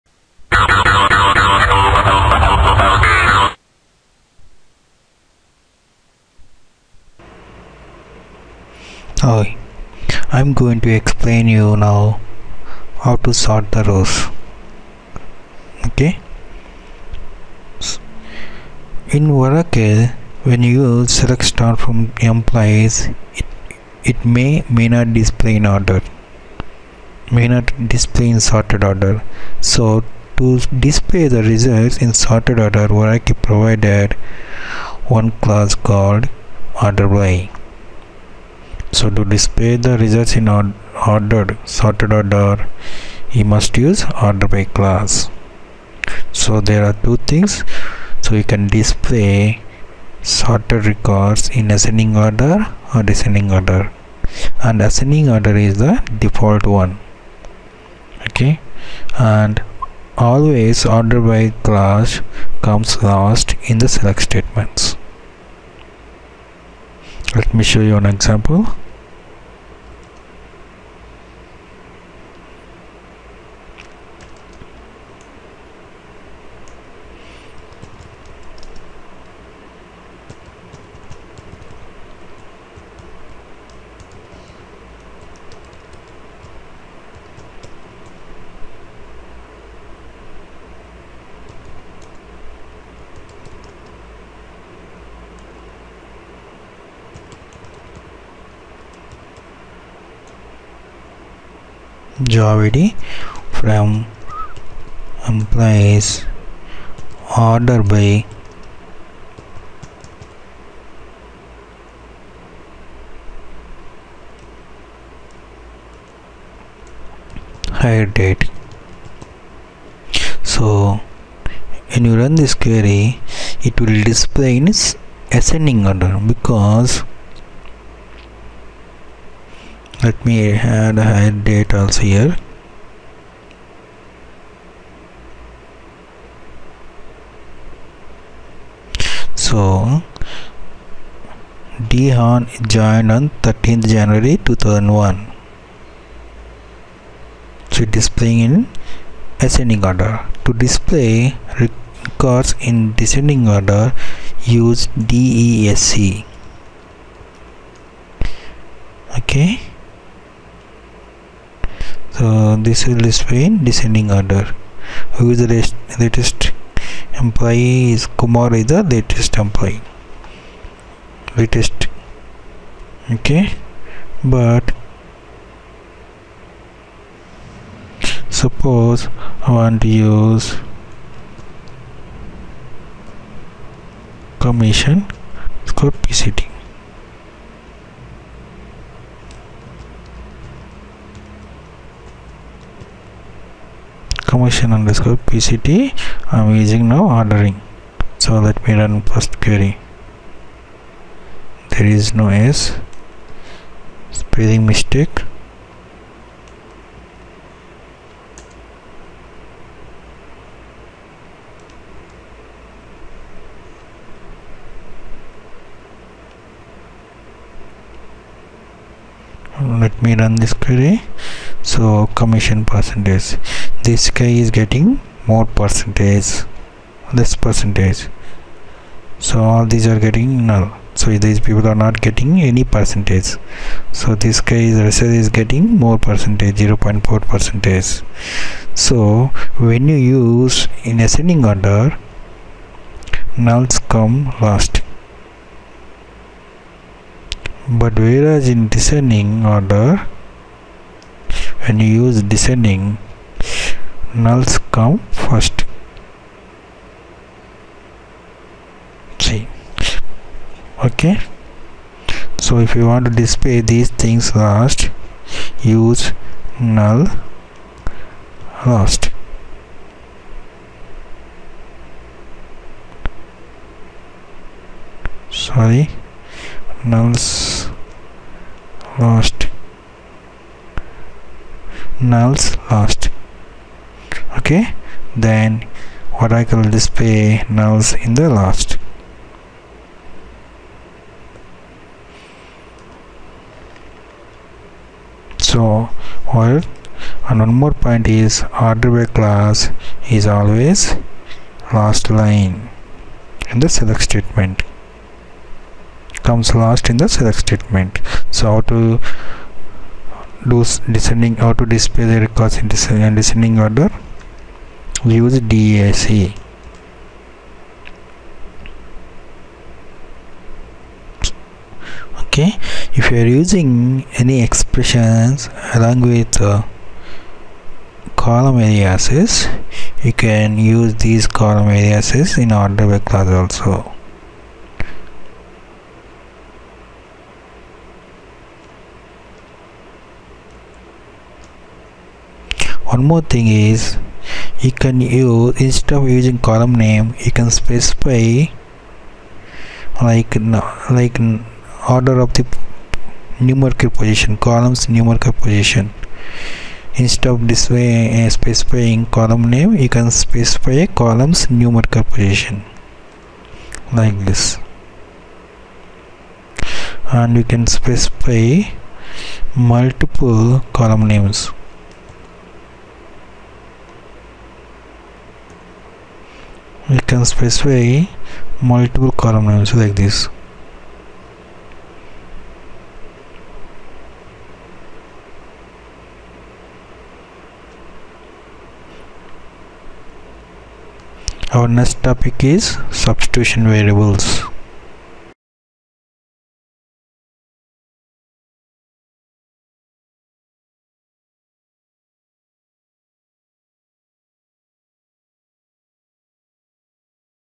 Oracle SQL Video Tutorial 13 : ORDER BY Clause
Oracle SQL Video Tutorials 13 : ORDER By Clause explained